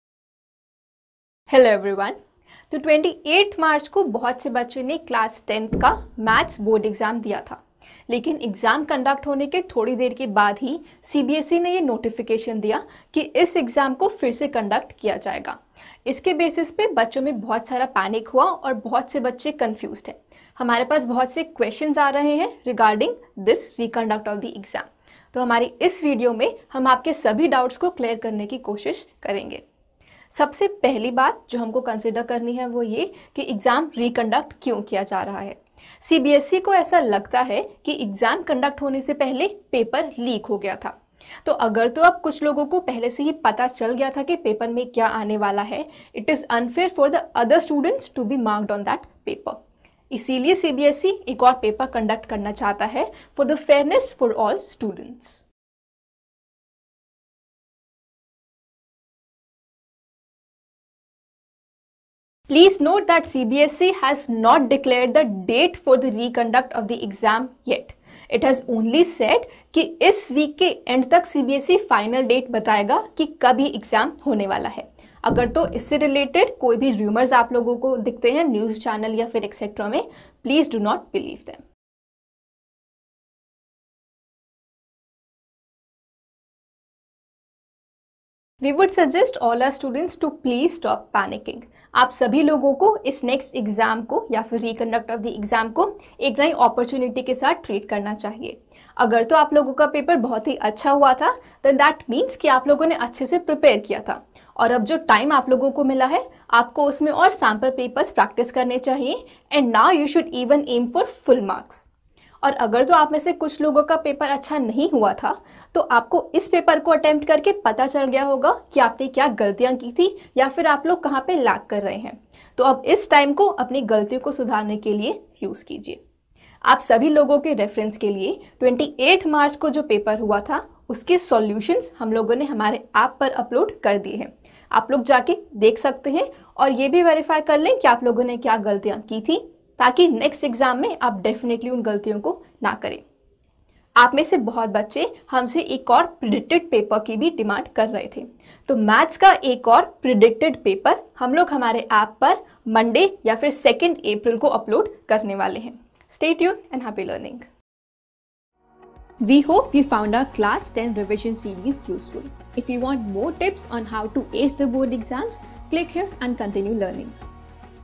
CBSE Class 10 Math Paper Solution | Predicted Paper by Avanti for Math Re-exam
A lot of students gave CBSE Maths board exam yesterday, but then CBSE released a notification about re-conduct of the exam.

In this video, we will clear all the doubts that students have!

First of all - why the re-exam?
CBSE doubts that the paper was leaked before the exam, and therefore it is unfair to go ahead with marking the paper for all students. Therefore, a re-exam will be conducted for the same.

When will the re-exam be conducted?
The exact date of re-exam has NOT been given by CBSE yet. Please do not believe any rumors that you might come across for the same. CBSE will decide the date for re-exam by the end of this week.

No need to panic!
Apart from this, we would suggest students to not panic. You should consider this as one more opportunity to perform better. If your first exam went well, great! It means you had prepared well and can now study even more and aim for full marks!
For students who couldn’t attempt the paper well, you should now take this time to work on your weak points for the next exam.

For all of your practice, we have released solutions of the paper conducted on 28th march on our app. You all should go through it and see what mistakes you made, and learn from them.

Also a lot of you have been demanding a new predicted paper from us, which we will be releasing on Monday, or 2nd april.